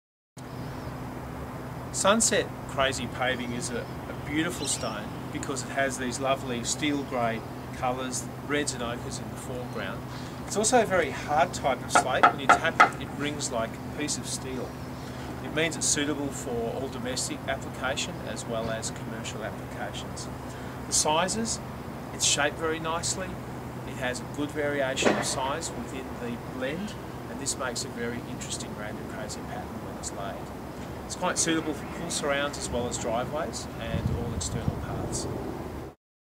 Edwards Slate & Stone - Sunset Random Crazy Slate
Random crazy pavers means the size varies according to how the stone was quarried.  The edges are usually hand cut,  however occasionally one edge can be sawn due to the extraction process.   Each random product is sized differently according to the quarries ability to produce ie. some random comes in large sizes and some the sizing is much smaller. The best way to view random is  ultimately  in the crates.  Keep in mind big pieces do not always mean better, as they are usually broken into smaller sizes by the fixer so that they match with the surrounding stone when laid.